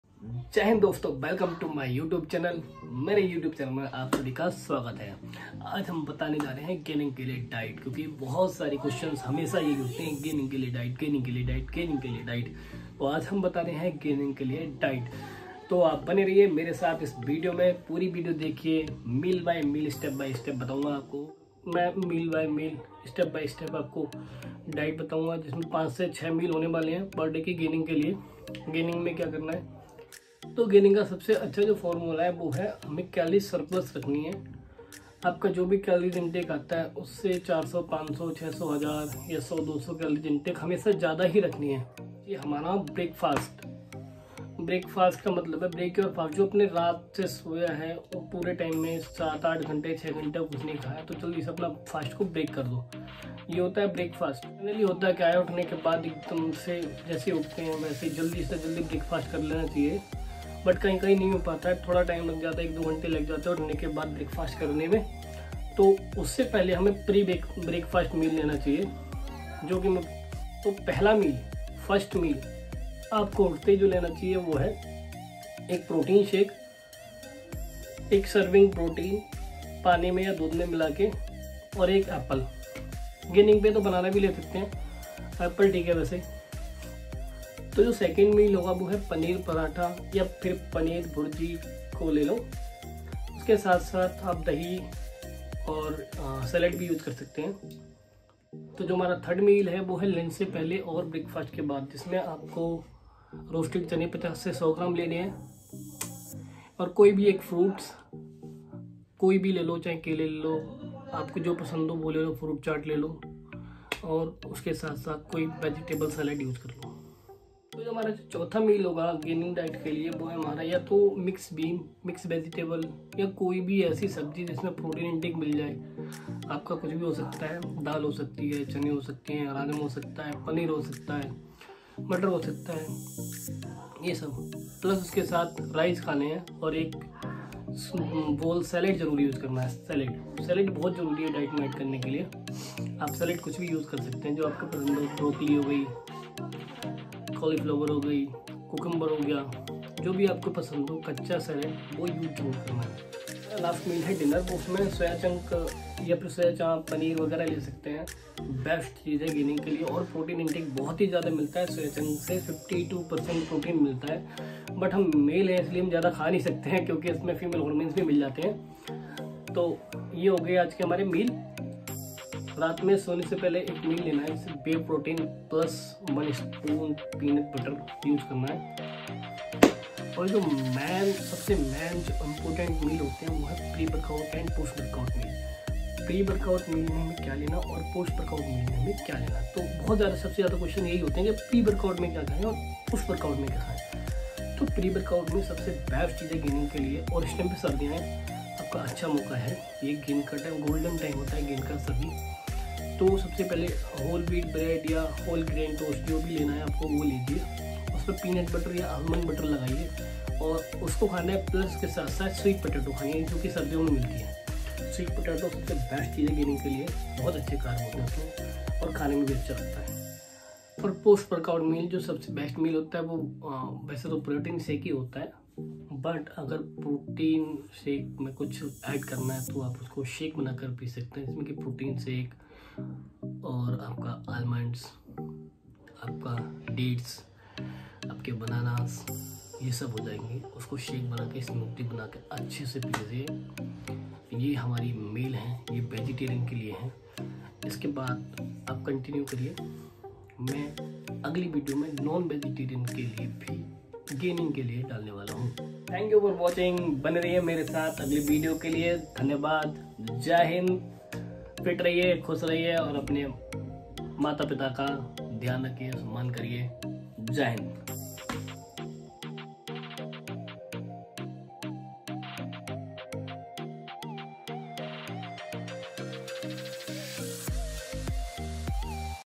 Vegetarian Diet Plan For Gaining
Comment for your topic

Wakeup Meal/ Pre-Breakfast

Whey Protein Shake and 1 Medium to large size of fruit

Breakfast
Paneer Parantha(3 medium peices)/Paneer Bhurji + Greek Yogurt/ Low-fat Yogurt + Sprout Salad(1 medium bowl)

Mid-Morning Meal/Snacks
Roasted Chickpeas(1 Medium Bowl) + Small size fruit(1 medium size) + mixed vegetables salad

Lunch
Mixed bean sabzi, Brown rice(1 medium cup) + Broccoli/Cauliflower(1 cup cooked)

Dinner
Avocado and White bean salad with cheese dressing

Pre-workout Meal
Whole Grain Toast(Spread almond butter or avocado) + Baked sweet potatoes(1 small bowl)

Post-Workout Shake
Whey Proten Shake and Dextrose Monohydrate